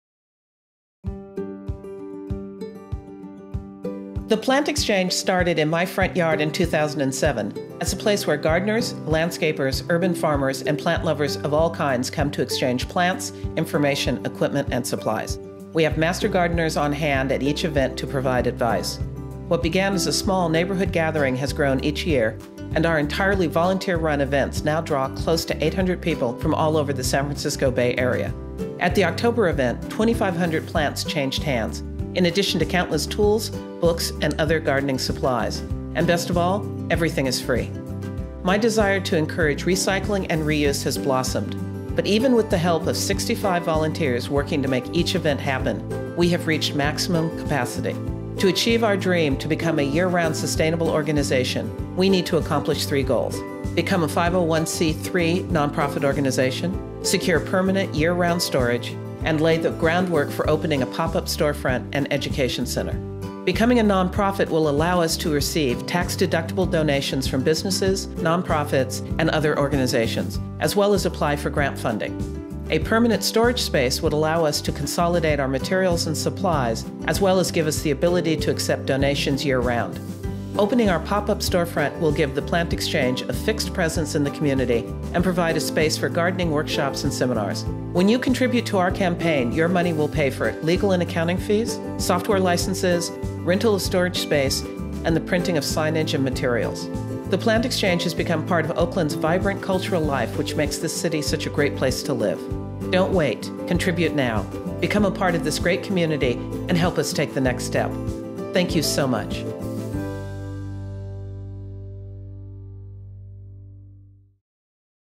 The Plant Exchange: IndieGoGo Campaign Video
The Plant Exchange is a free biannual community event in Oakland, California which encourages reuse, recycling, organic living, resource sharing, and information exchange. Neighbors, gardeners, landscapers, urban farmers, and outdoor fans come from all over the San Francisco Bay Area. On average, we draw 700 participants and exchange 2,500 plants. Every year we grow, and we have reached our limits.

That’s why we’re launching an IndieGoGo Campaign! We need $12,000 to incorporate as a 501c3 non- profit organization, secure a permanent home, and become self-sustaining.

If you value these events and have a few dollars to spare, please make a donation today!